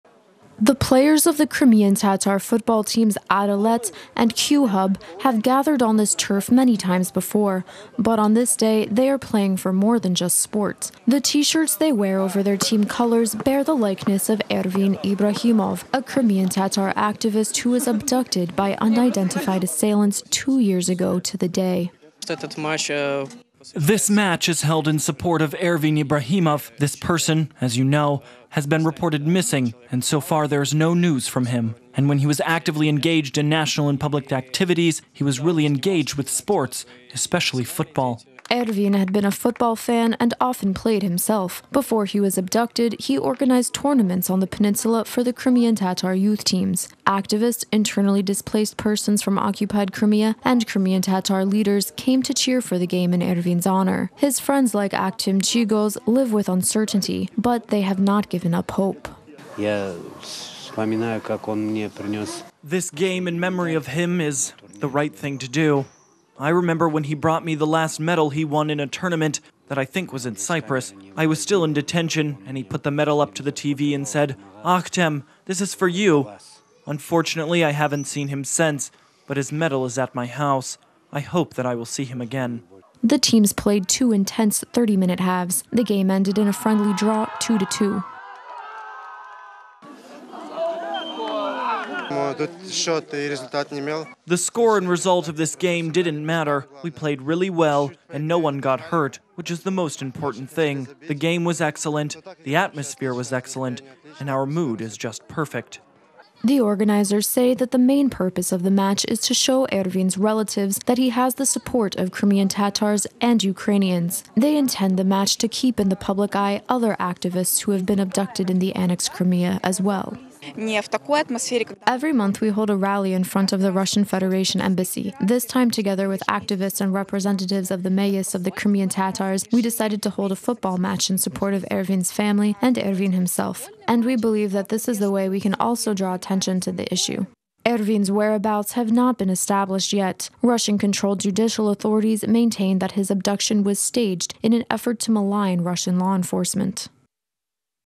Crimean Tatars Meet on the Field in Support of Abducted Tatar Activist
Activist, member of the World Council of Crimean Tatars, and role model -- this is what the Crimean Tatars lost on May 24th, 2016, when Ervin Ibrahimov was abducted. On the 2nd anniversary of his abduction, 2 Crimean Tatar football teams this weekend took to the field in support of Ervin and his family.